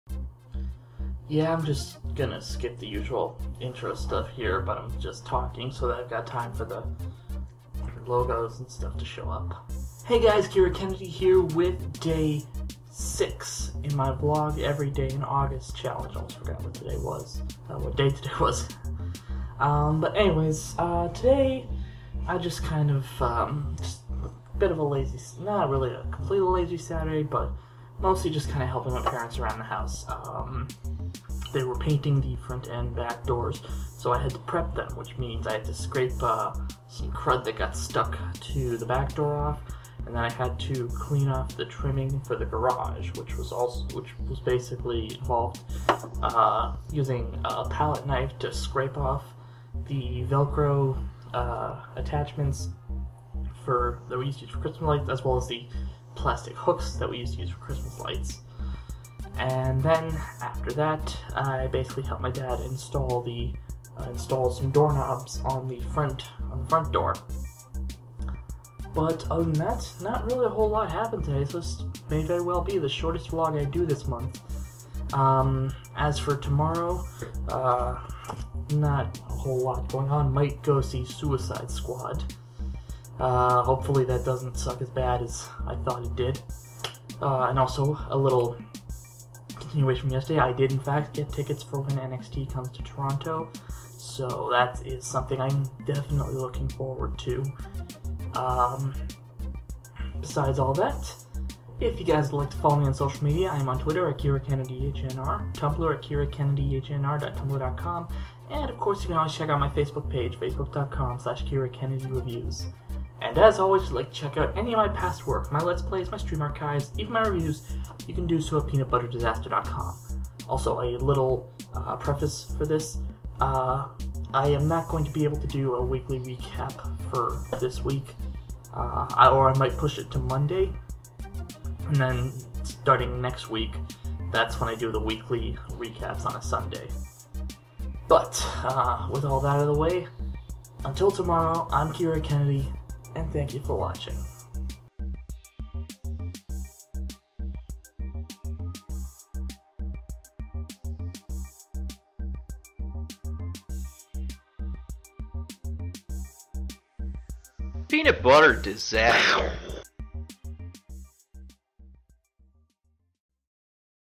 KiraKennedy VEDA 2016 (Part 2: Electric Boogaloo) - Day 6
Helped out with some minor renos around the house today